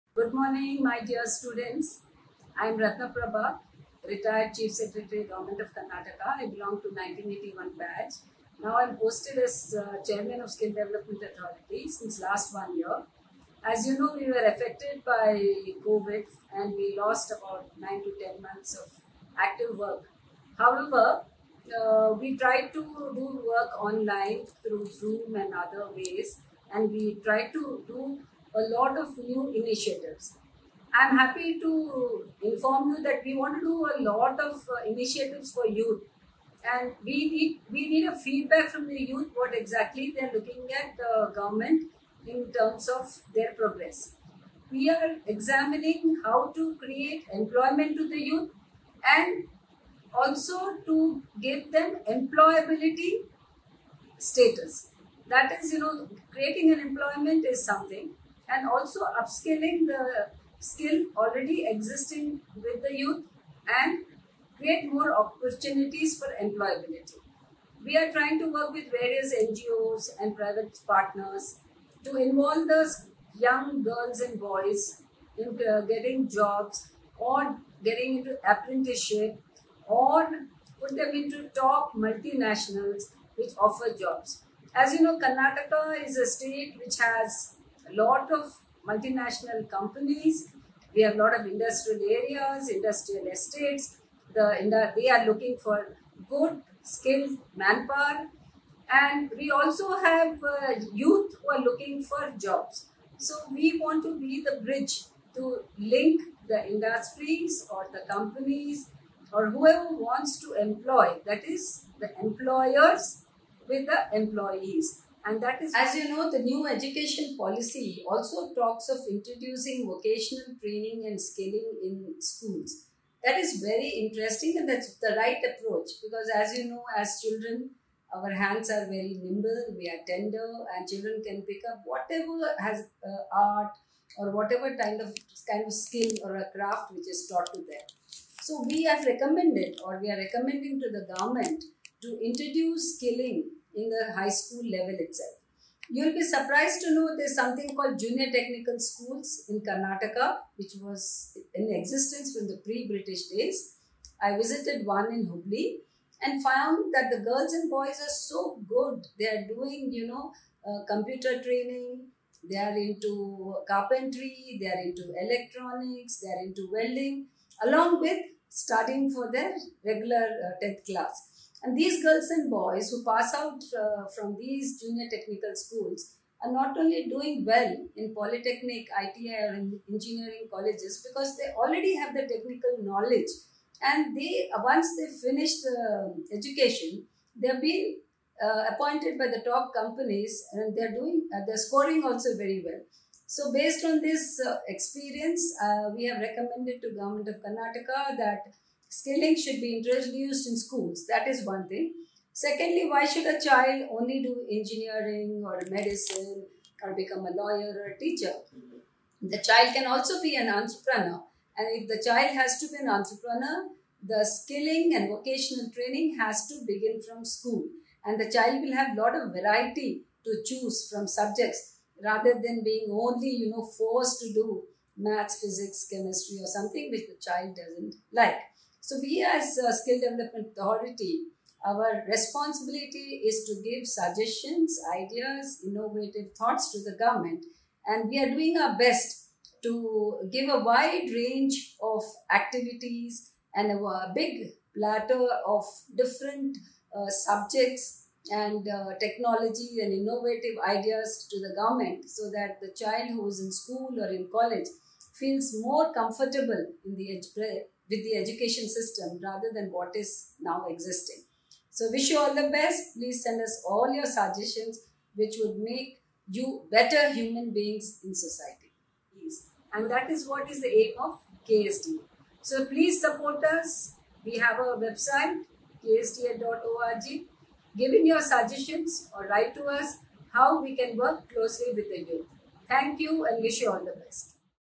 K_Ratna_Prabha Chair person of Karnataka Skill Development Authority
The State Government constituted the Karnataka Skill Development Authority under the Department of Skill Development Entrepreneurship and Livelihood, Government of Karnataka to establish quality standards in skills training, prepare curriculum, course, content and credit frameworks for skills training, prepare guidelines for training trainers, prepare standards for skill training and to improve the standards of the skill programmes in the State, design and to develop labour market information systems and data systems based on global standards and best practices, to work as a think-tank forum for research inputs relating to skills development and to take up other activities in order to ensure overall improvement in Skills Training and furthering the employment opportunities to the youths_of_Karnataka.